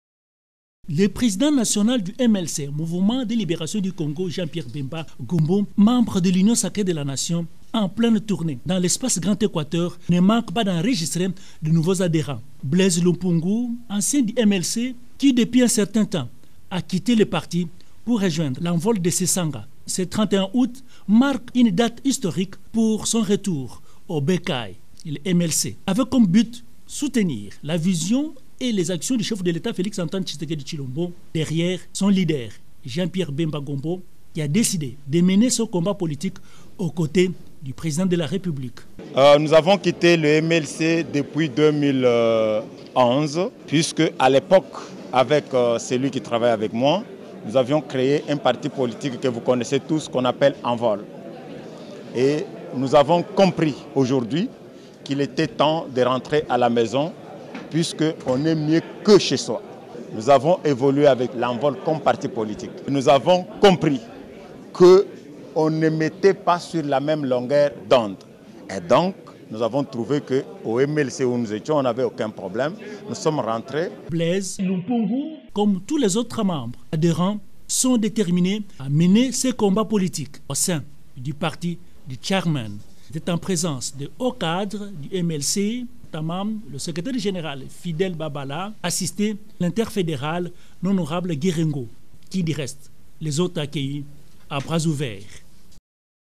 ENVOL DE RETOUR AU MLC, BLAISE LUPUNGU, BUBIS BUNZIKA BISAMBU, EDDY MALENGA, AUGUY LUKENGO.
un accueil chaleureux des cadres du MLC réservé aux ex-cadres de l'Envol.